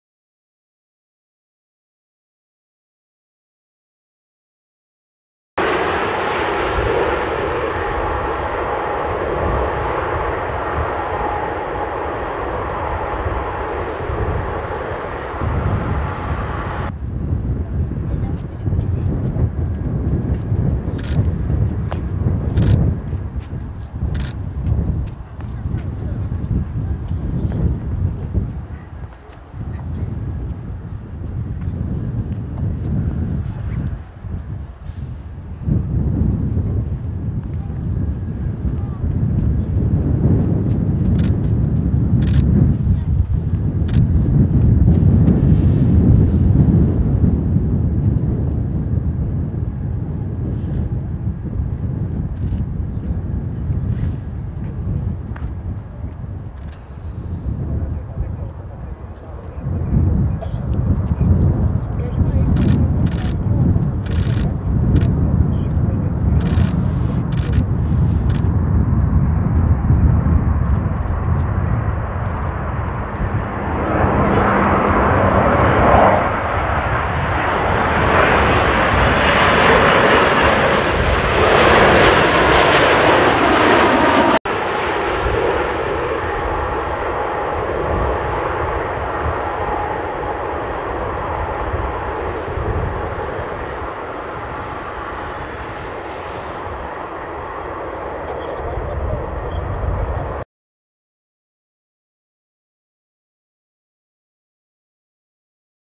☆エティハド航空 Boeing 787-9Dreamliner Landing Narita RWY34L成田空港!ひこうきの丘
エティハド航空 (Etihad Airways)
Boeing 787-9 Dreamliner
A6-BLE